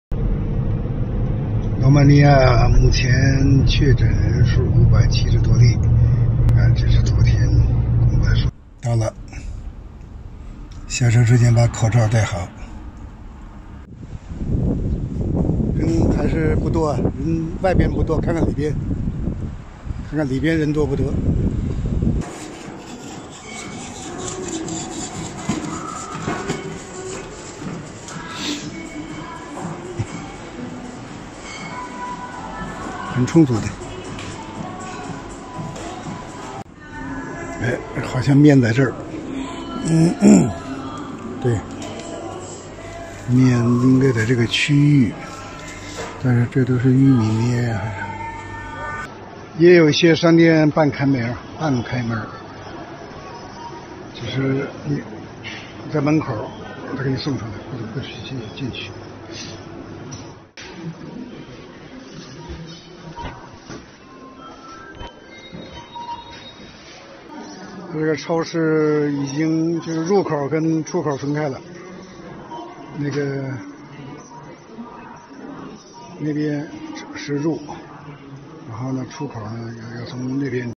Vlog: 疫情之下的罗马尼亚生活
截至3月24日，罗马尼亚的确诊人数有570多例。李建华，一位当地华人报纸的主编将为我们介绍疫情之下的罗马尼亚当地的生活状态。